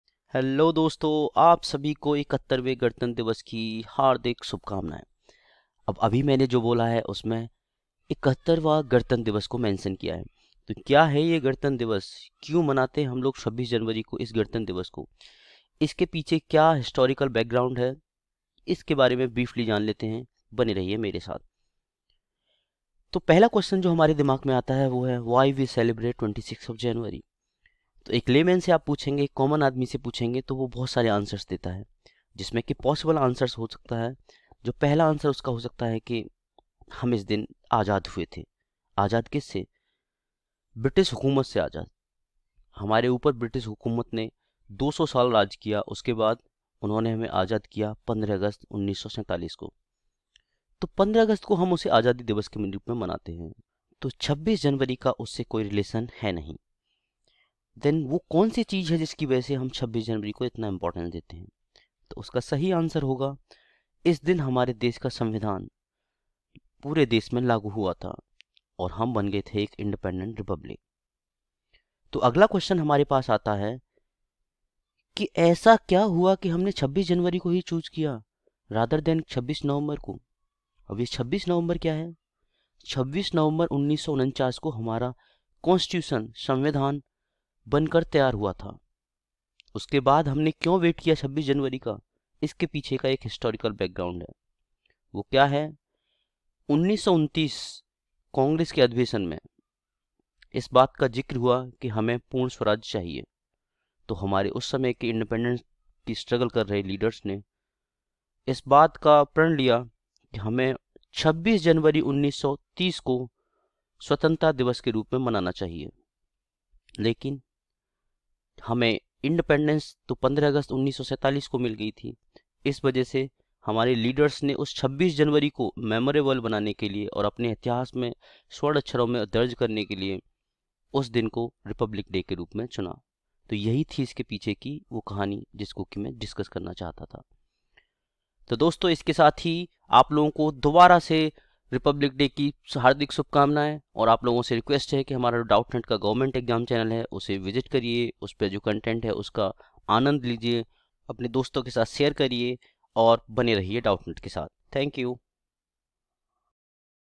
हम 26 जनवरी क्यों मनाते हैं? | 71st Republic Day | Republic Day 2020
Why do we  celebrate 26 January every year?